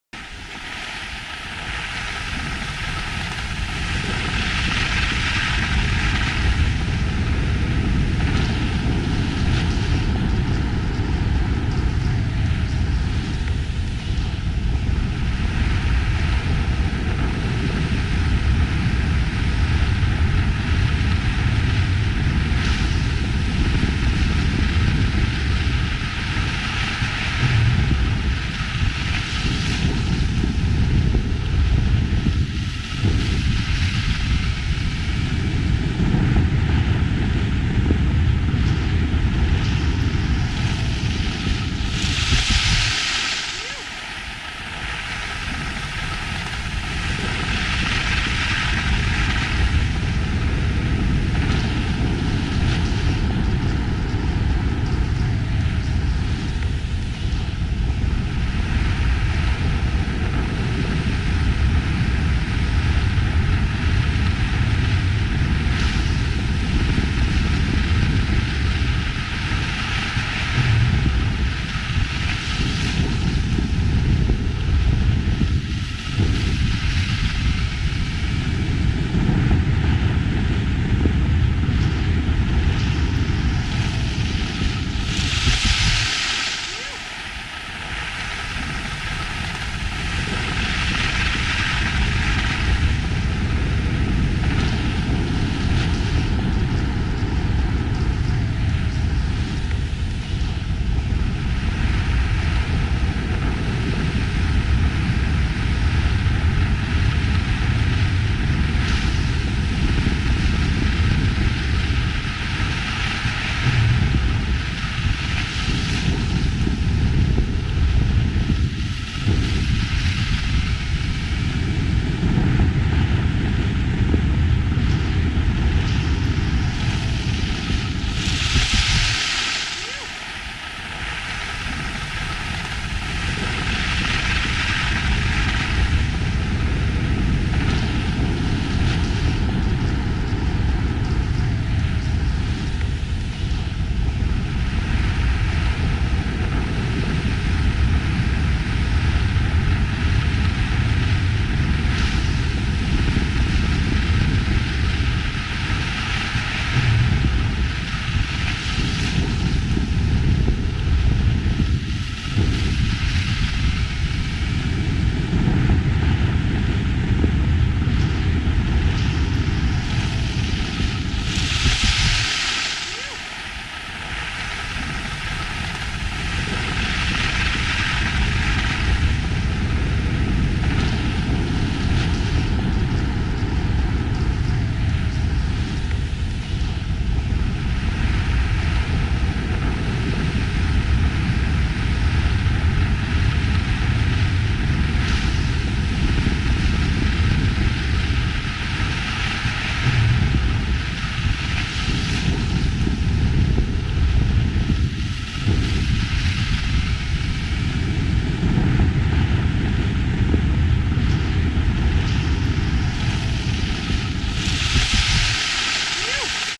Pista Supertrail a Thredbo Resort in Australia 🇦🇺 ⛷
Sequenza  ipnotica 😀 della pista Super Trail che si trova a Thredbo Resortin Australia ieri. neve molto buona.
Siamo nel Galles del Sud, scoprite qui tutto su questa stazione sciistica Australiana